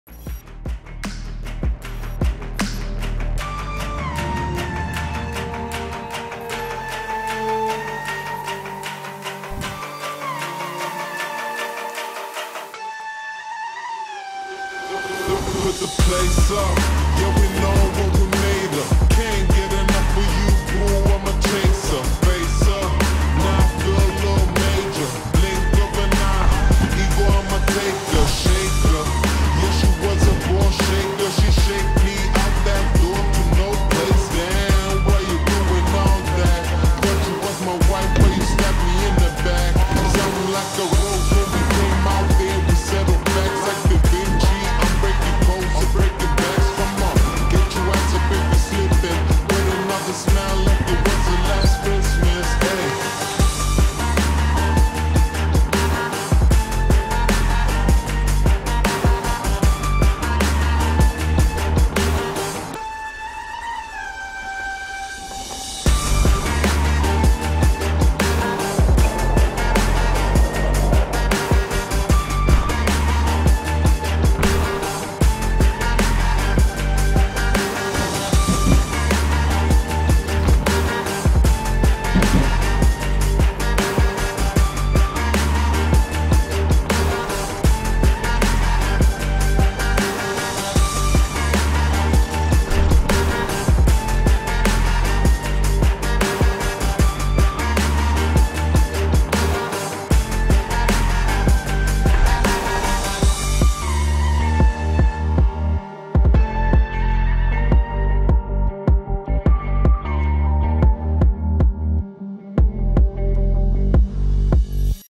Martin Matejovie MTB
A quick edit we've put together to showcase the amazing talent of this young rider.